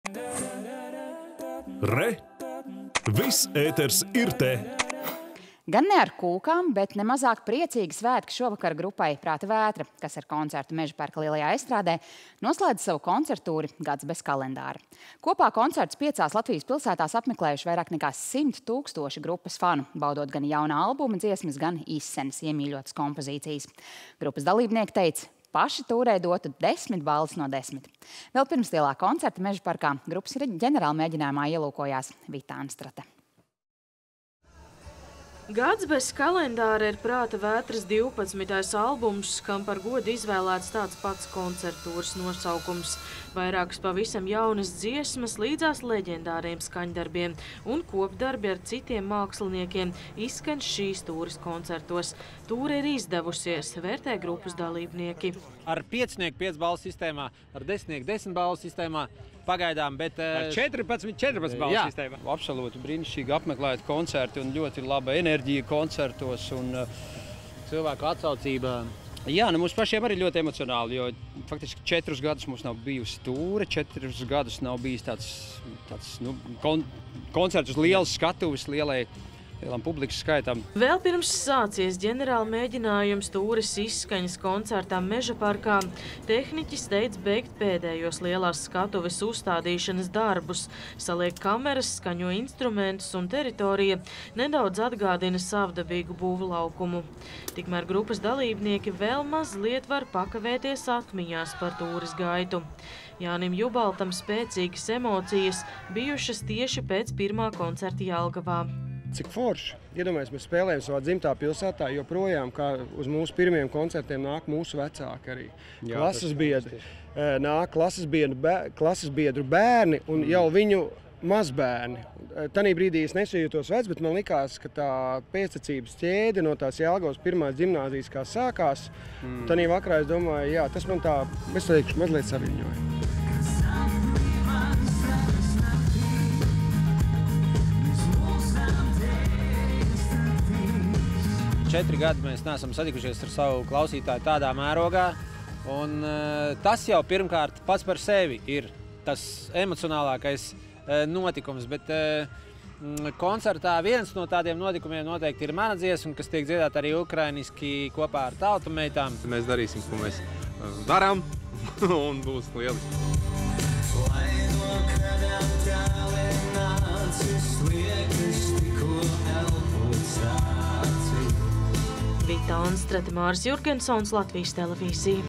Noslēdzas “Prāta Vētras” koncerttūre “Gads bez kalendāra”
Ar koncertu Mežaparka Lielajā estrādē šovakar noslēdzas grupas “Prāta Vētra” koncerttūre “Gads bez kalendāra”. Kopā koncertus piecās Latvijas pilsētās apmeklējuši vairāk nekā 100 000 grupas fanu, kas varēja baudīt kā jaunās albuma dziesmas, tā arī izsenis iemīļotas kompozīcijas. Grupas dalībnieki teic, ka viņi paši tūrei dotu desmit balles no desmit! Bet vēl pirms lielā koncerta Mežaparkā, grupas ģenerālmēģinājumā ielūkojās Vita Anstrate.